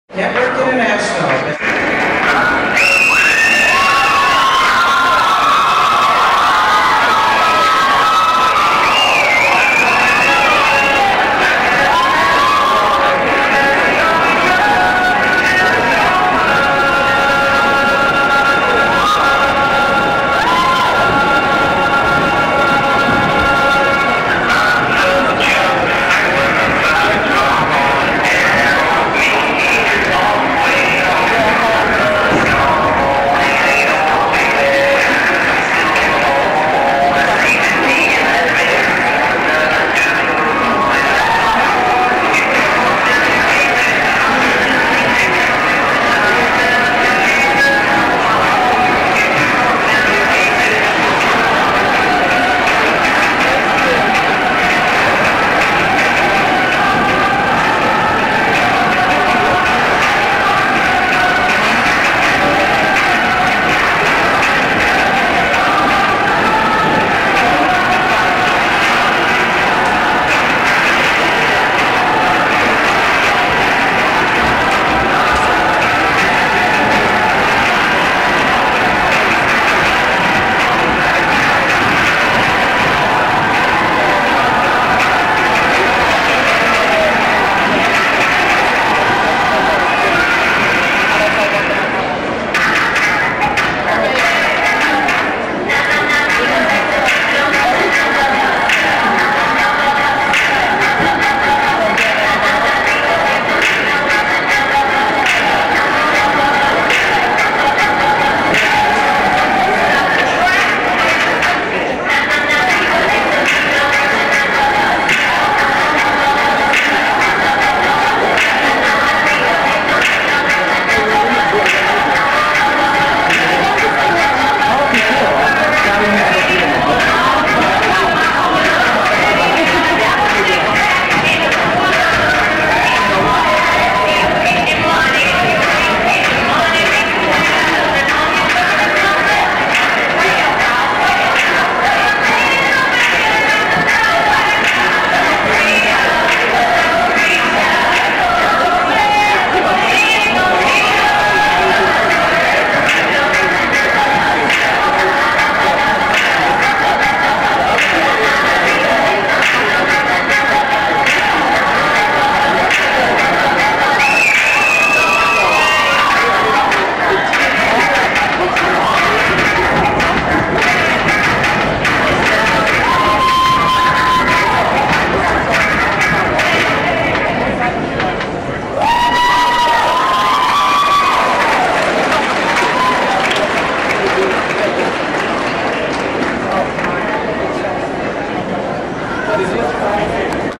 IMTA NY 2009 GROUP RUNWAY
Network International Models & Talent ROCKED THE RUNWAY the runway at the New York 2009 IMTA Convention. Our theme this year was "St. Patrik's Charm School".
Catholic school will NEVER be the same!